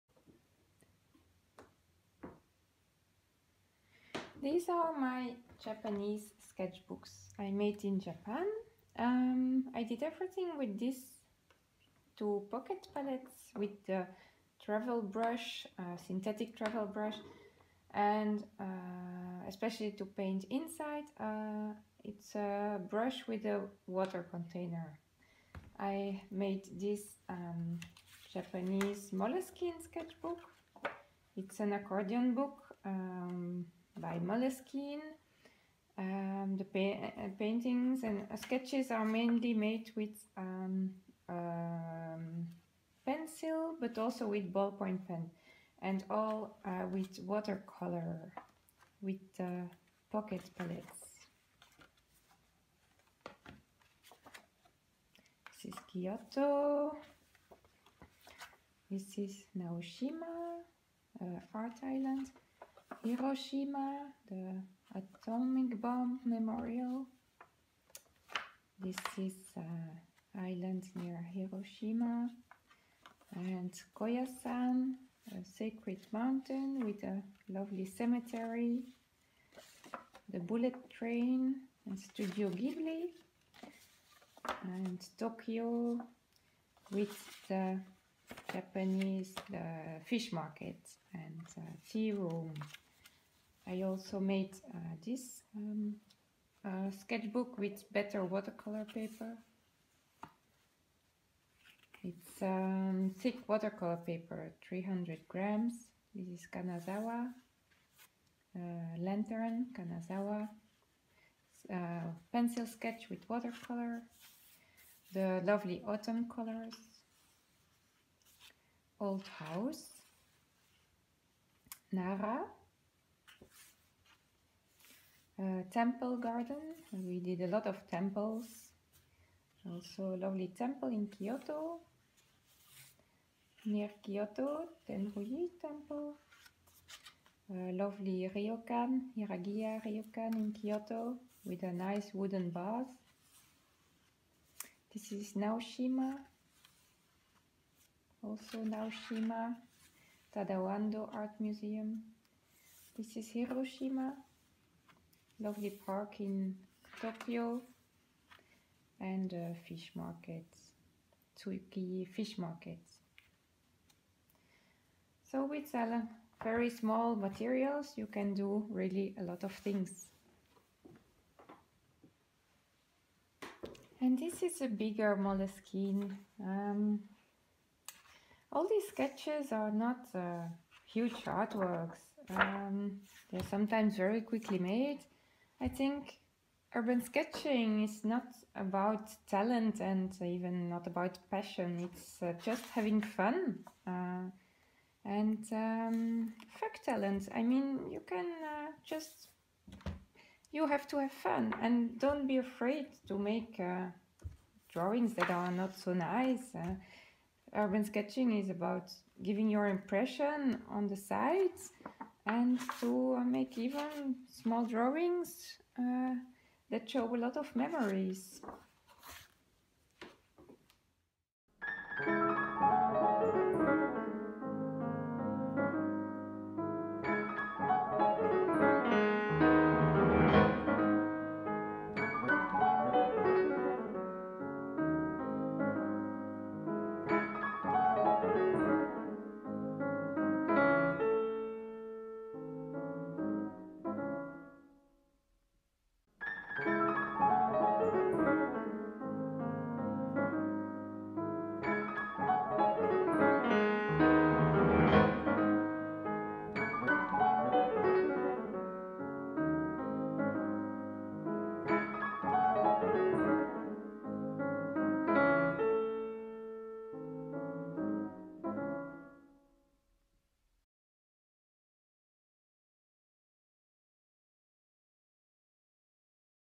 Urban sketching in Japan
This video is about Urban sketching in Japan

I sketch with watercolour, pencil and ballpoint pen in Moleskine sketchbooks.